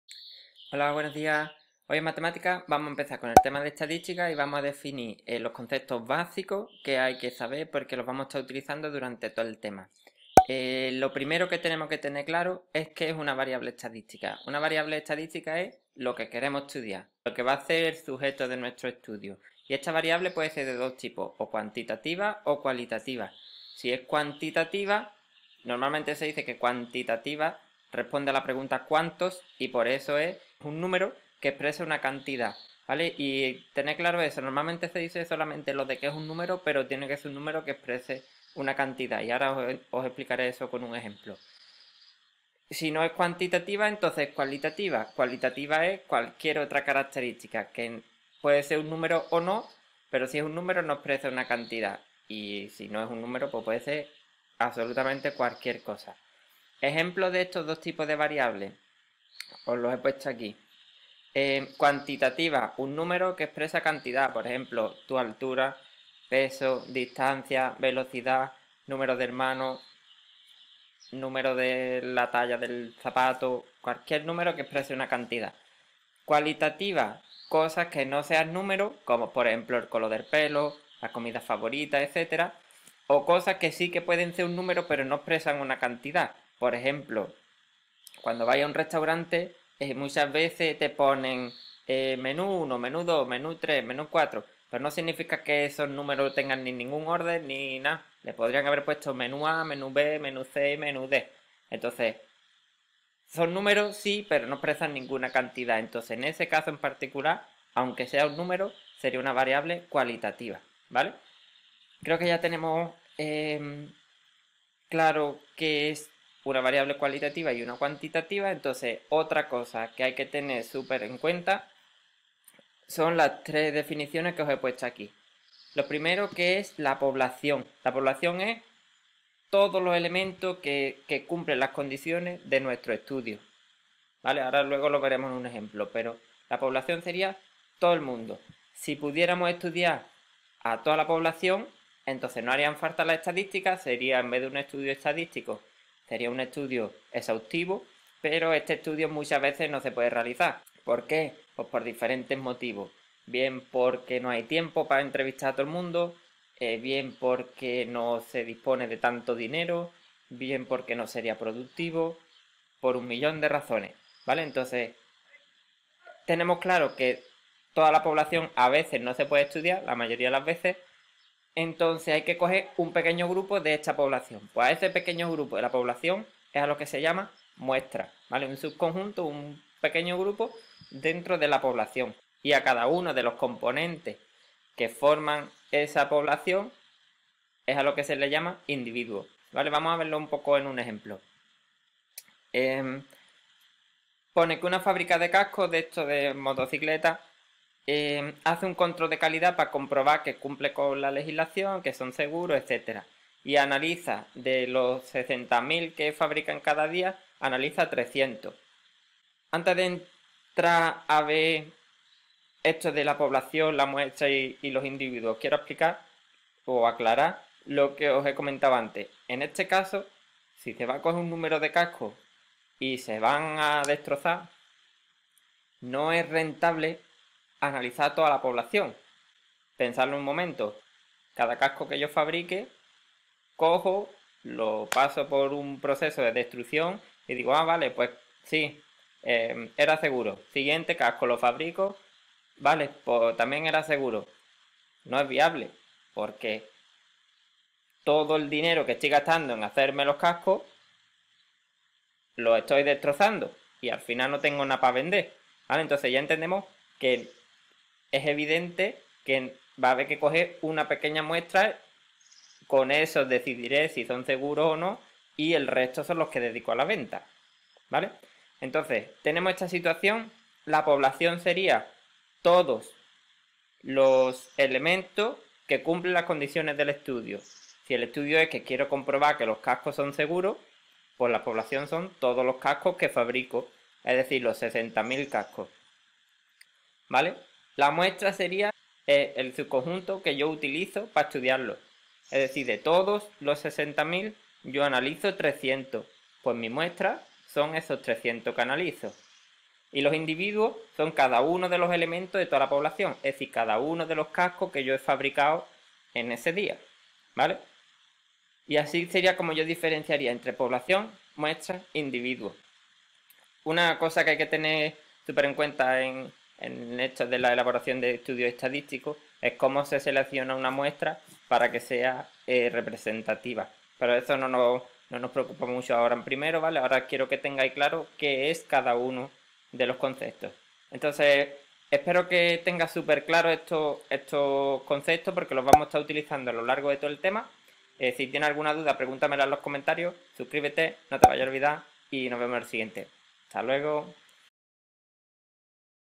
Conceptos básicos de estadística [#1 Estadística] [1º ESO]
En este vídeo aprendemos los conceptos básicos que trabajaremos a lo largo de todo el tema de estadística:
- Variable, variable cuantitativa y cualitativa.
- Población, muestra e individuo.

¡Aquí me puedes parar, darle para atrás y ponerme tantas veces como quieras o lo necesites!

Déjame tus dudas o lo que te ha parecido en comentarios.